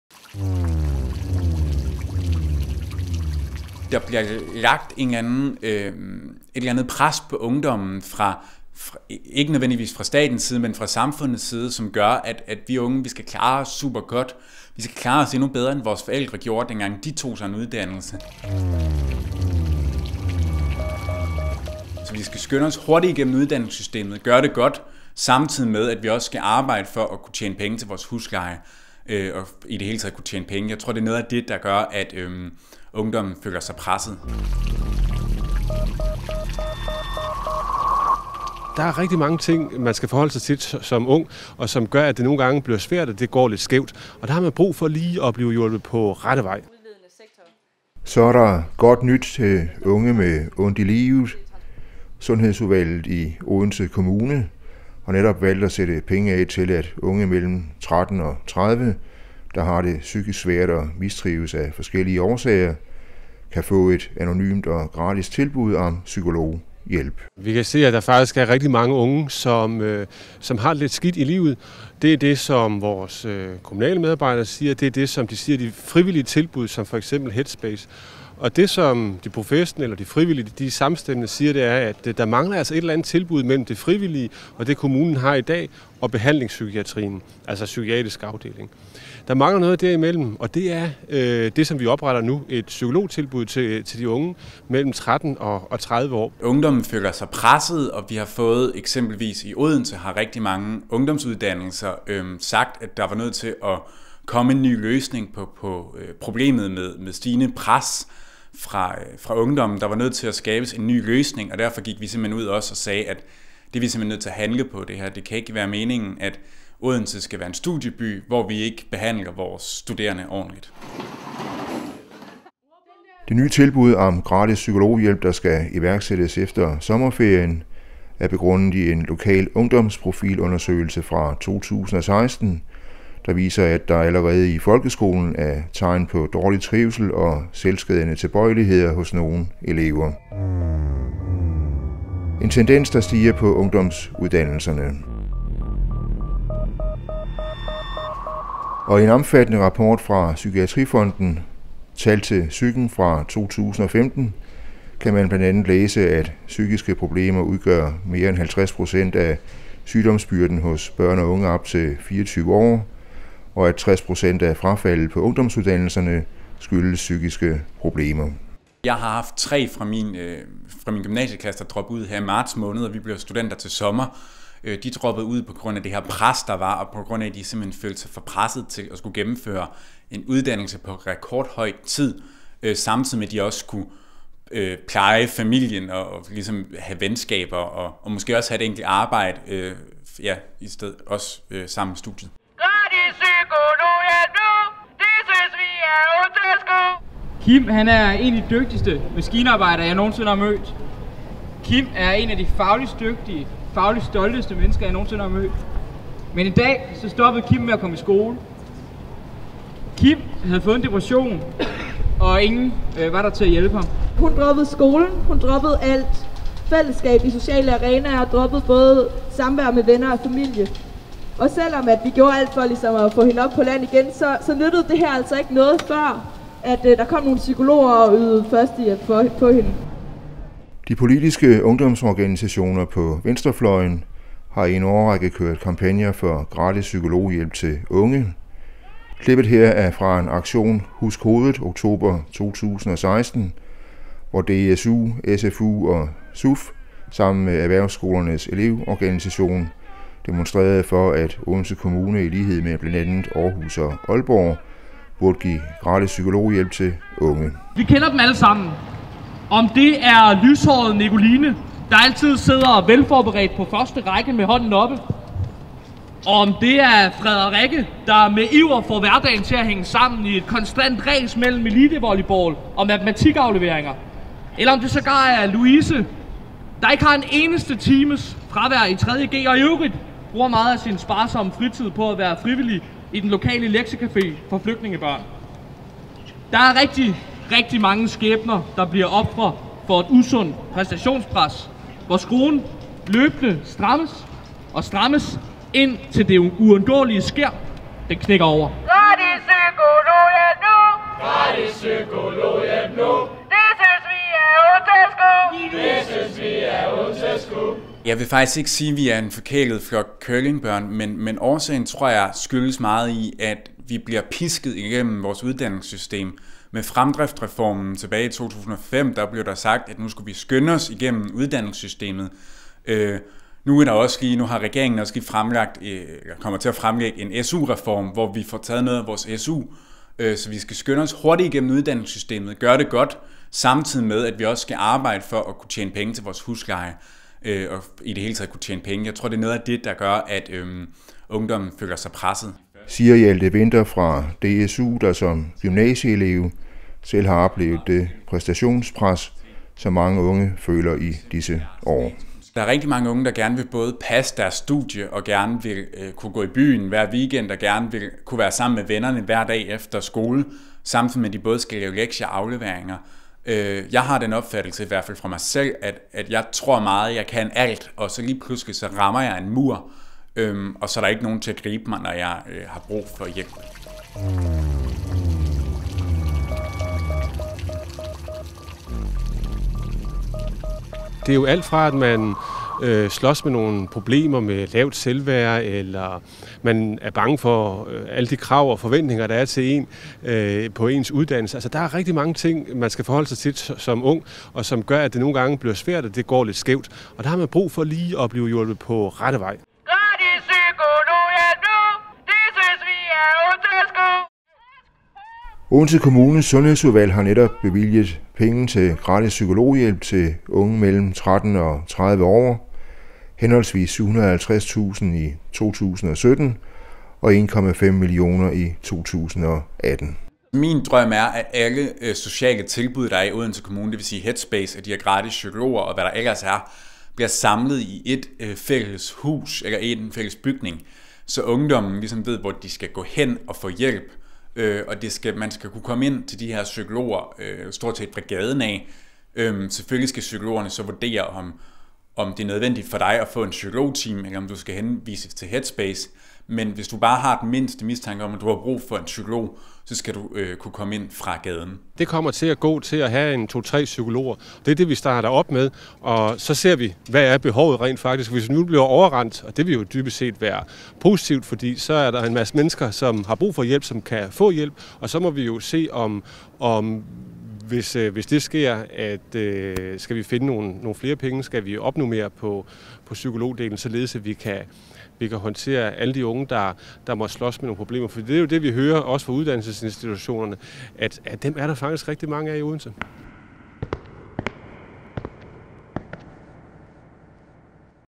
Godt nyt til psykisk sårbare unge.
Så er der godt nyt til unge med ondt livet. Sundhedsudvalget i Odense Kommune har valgt at sætte penge af til at unge mellem 13-30, der har det psykisk svært og mistrives af forskellige årsager kan få et anonymt og gratis tilbud til psykologhjælp.
Vi har mødt formanden for udvalget, Brian Dybro SF og DSU'eren Hjalte Vinter, der giver nogen bud på hvorfor flere unge får psykiske problemer og dropper ud af deres uddannelse.